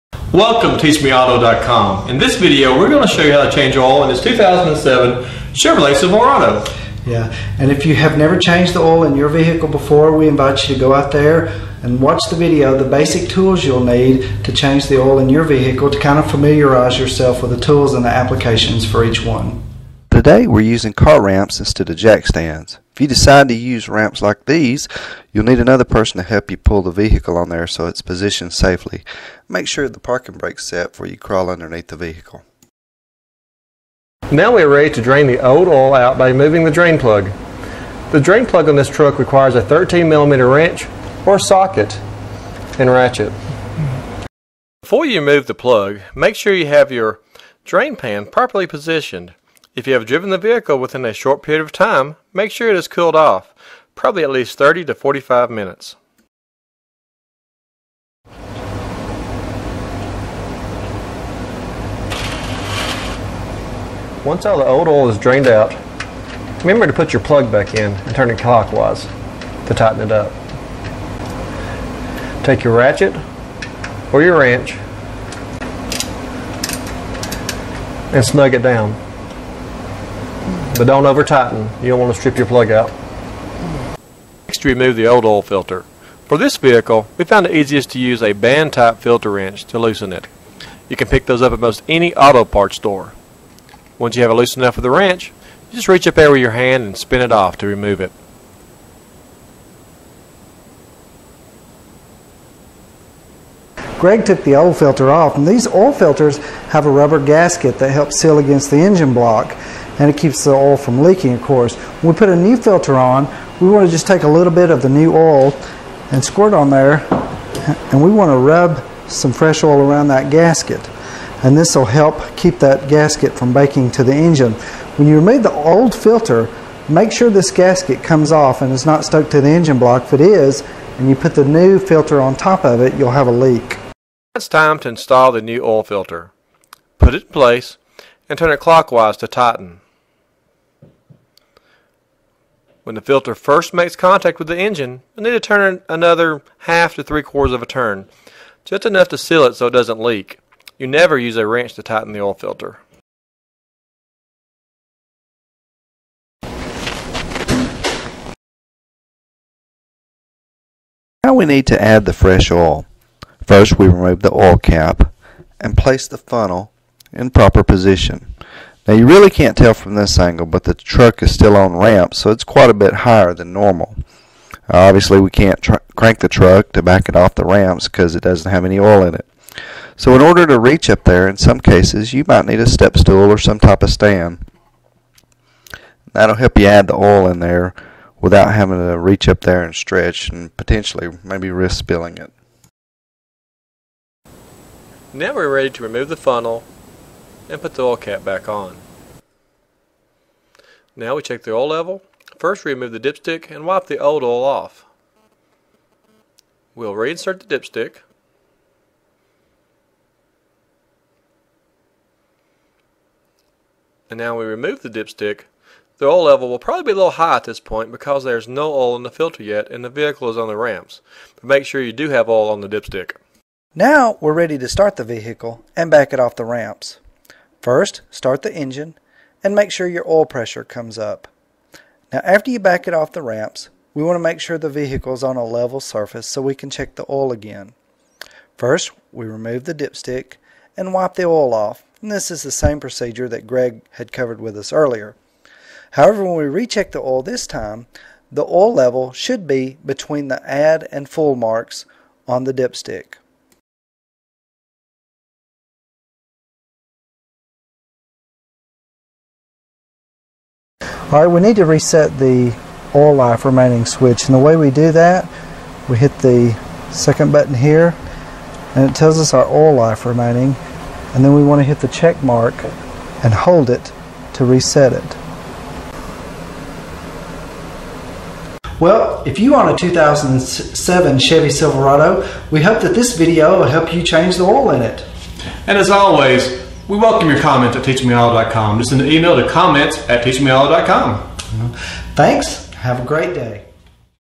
Oil Change 2007 Chevy Silverado 1500
In this video the guys will show you how to change the oil, filter and reset the oil life remaining switch in a 2007 Chevrolet Silverado 1500 pickup